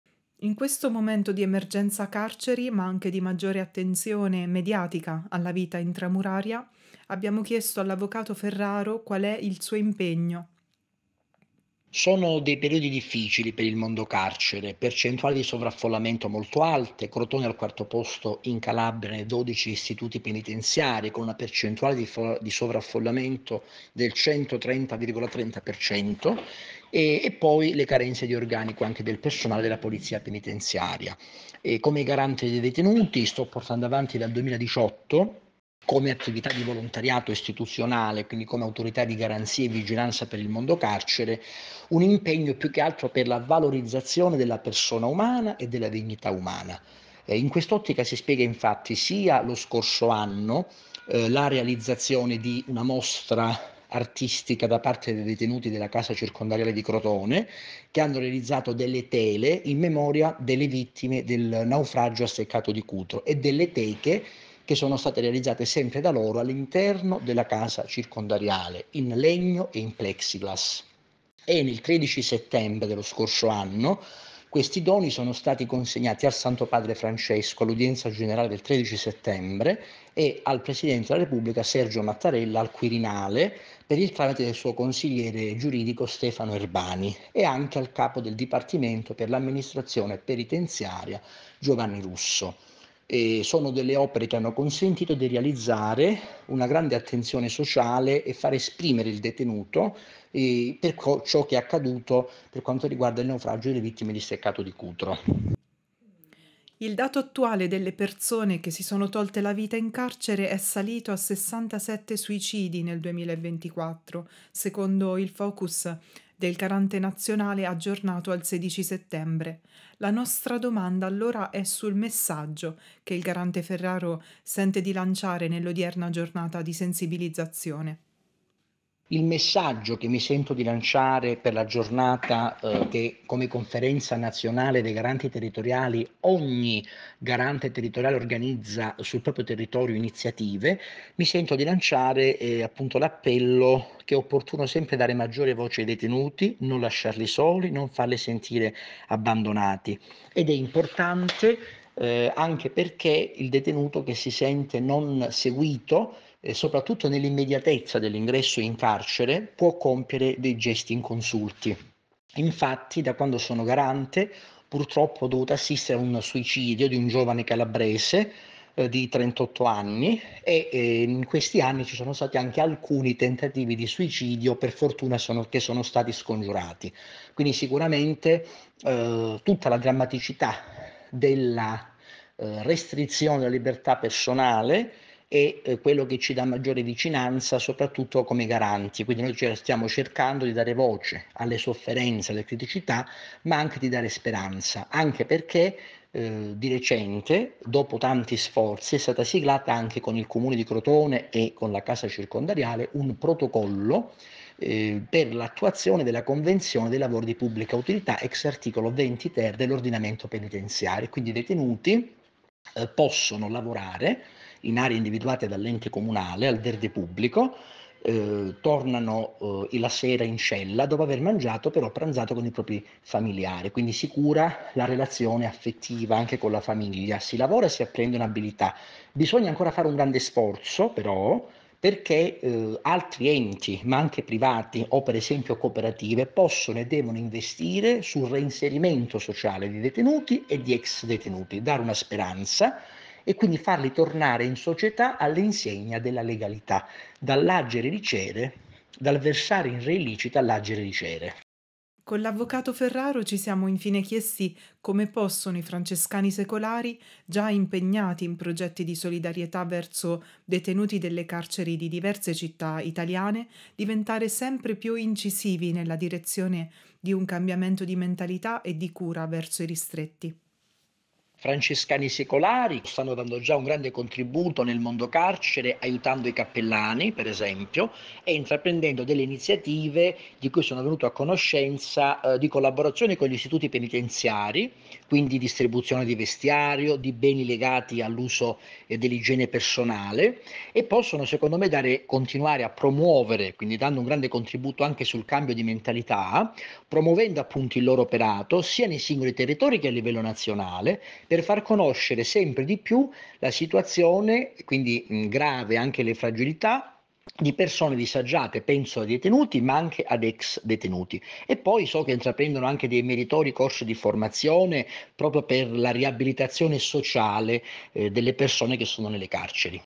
INTERVISTA CON FEDERICO FERRARO
Il Garante dei detenuti di Crotone riporta il suo impegno e lancia un appello per fare degli istituti di pena luoghi di umanità e speranza